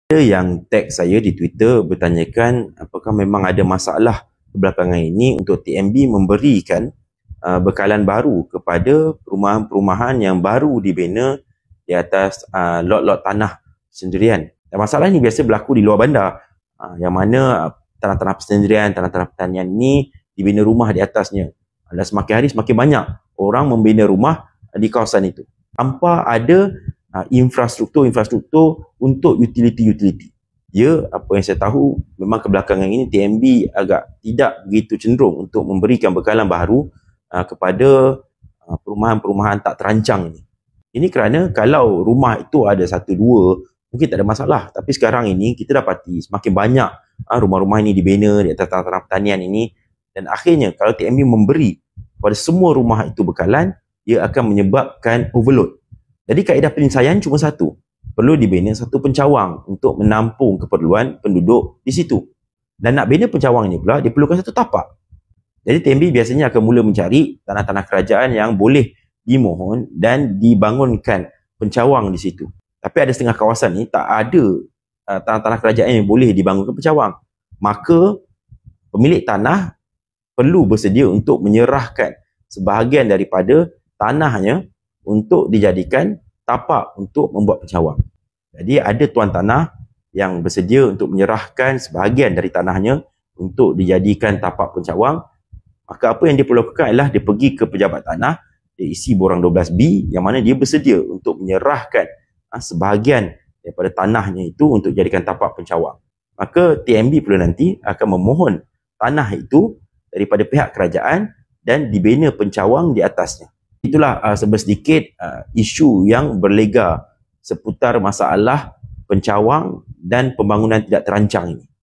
Isu kawasan : Pencawang TNB - Edry Faizal (ADUN Dusun Tua)
Isu kawasan : Pencawang TNB

Berkenaan bekalan baharu TNB dan pembinaan pencawang bagi menampung permohonan baharu.